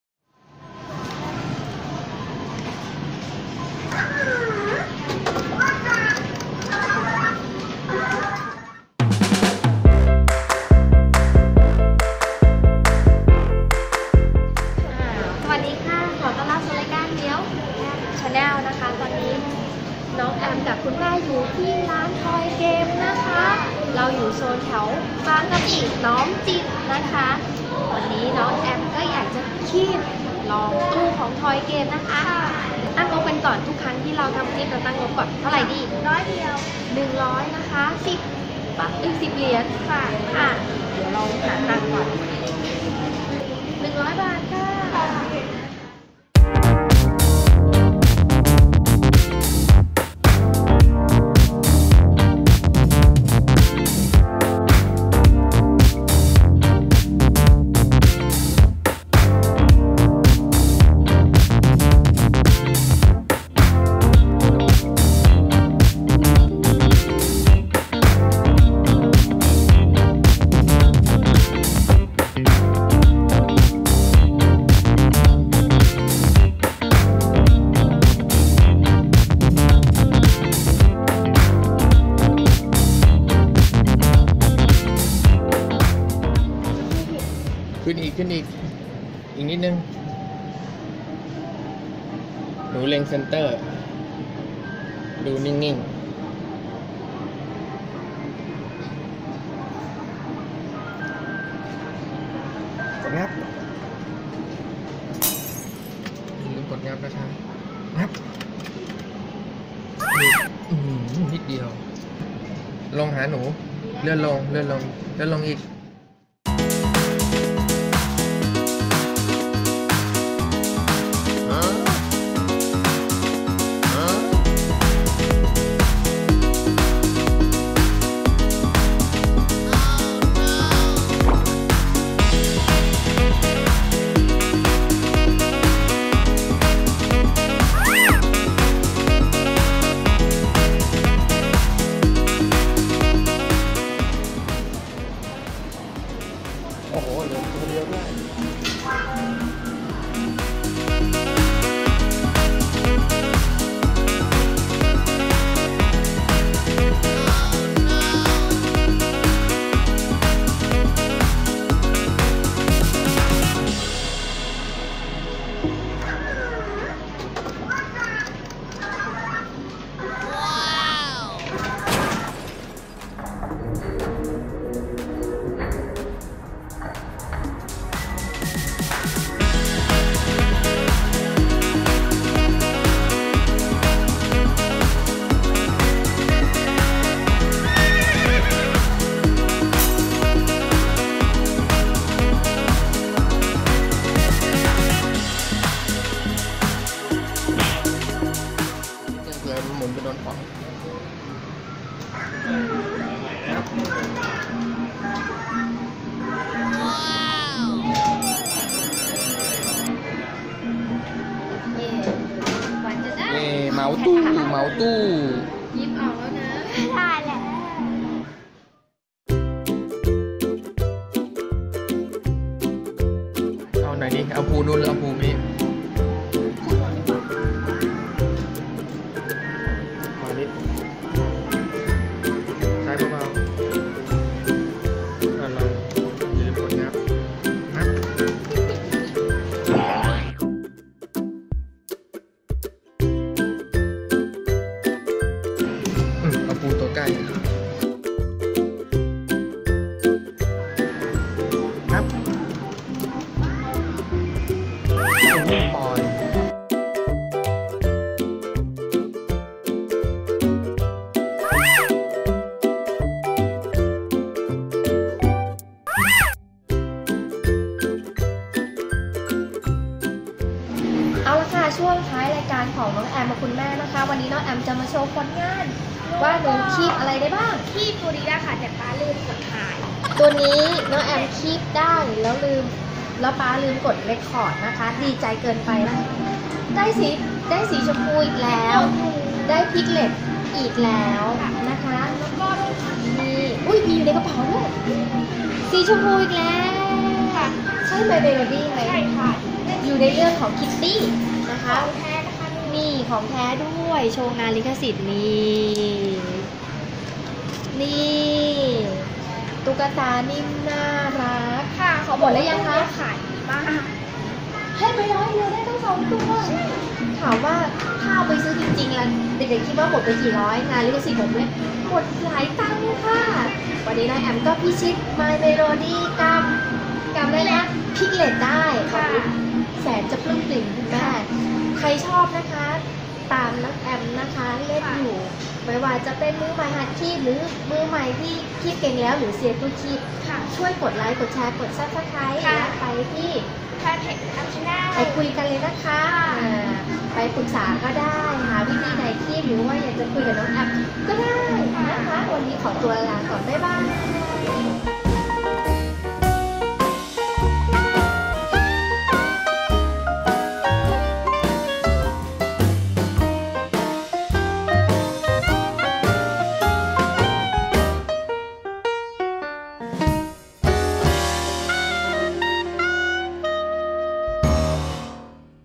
คีบตุ๊กตา ที่ไหนดี? ร้าน Toys game สาขา N-mark Plaza (น้อมจิตต์)
ขอต้อนรับทุกท่านสู่ Maew Amp Channel คะ
วันนี้น้องแอมป์กับแม่เหมียวจะมาแนะนำร้านคีบตุ๊กตาในโซนบางกะปิอีกหนึ่งร้านคะ ร้าน Toy Game สาขานี้ตั้งอยู่ที่ชั้น 3 ห้าง N-Mark Plaza หรือห้างน้อมจิตต์นั้นเอง อยู่ตรงข้ามเดอะ มอลล์ บางกะปิ ใกล้ใครก็ลองแวะมาสนุกกันได้นะคะ